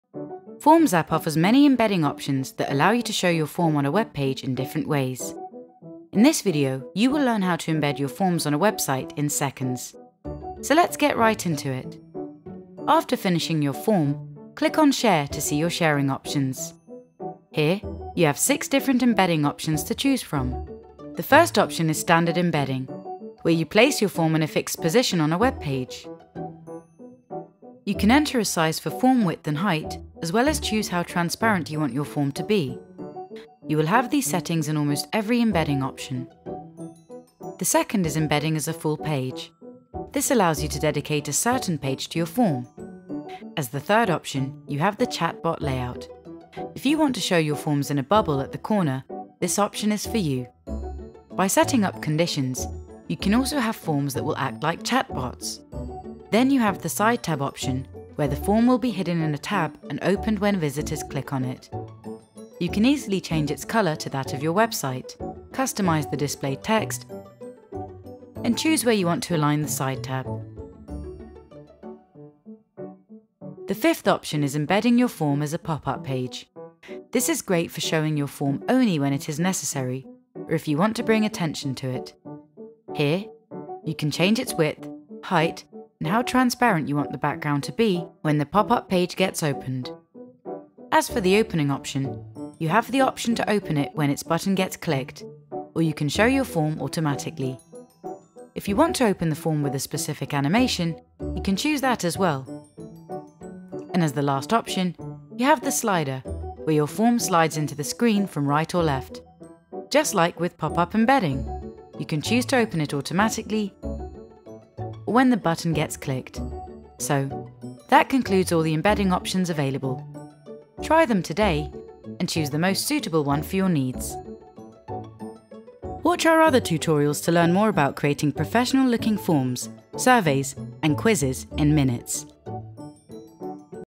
How to embed a form into a web page
Embedding a form on your website is a great way to get people to sign up for your email list, register for an event, or even purchase a product. In this video, we show you how to get and use the embed code of your form.

00:00 Introduction
00:12 How to embed a form into a web page
02:05 Subscribe